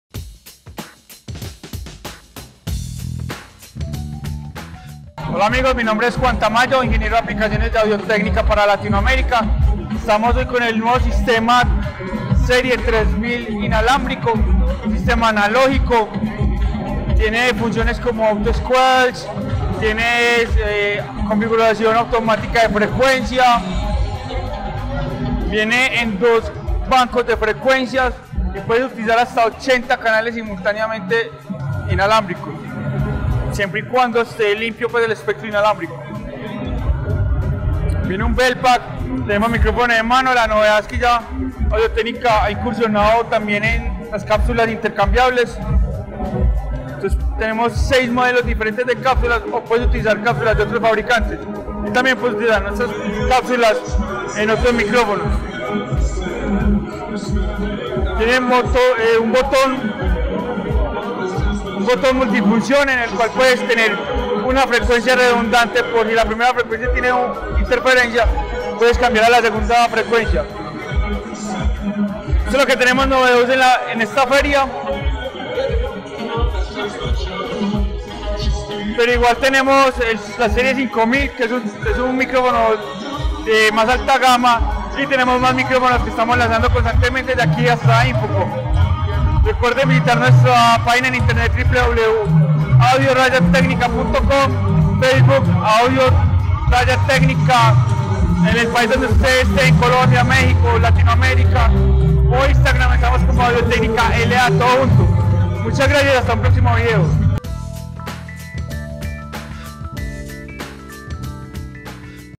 Visita a Audio Technica durante Sound:Check Xpo 2018
Entrevista con Juan Tamayo
Empresa: Audio Technica
Realizada por Richard Santa
Evento: Sound:Check Xpo
Abril 2018